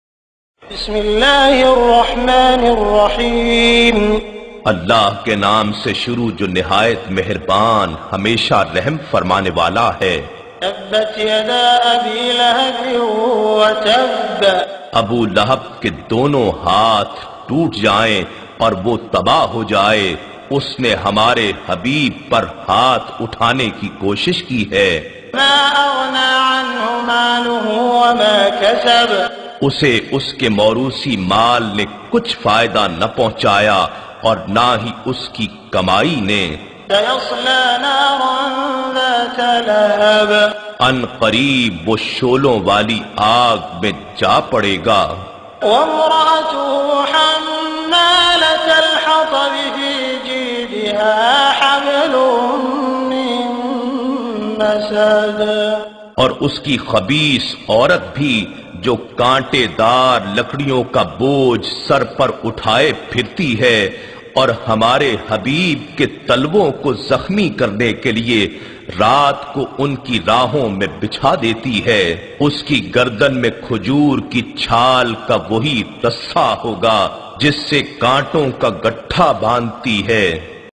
111-Surah Lahab
111-Surah Lahab-Allah's curse to Abulahab & his wife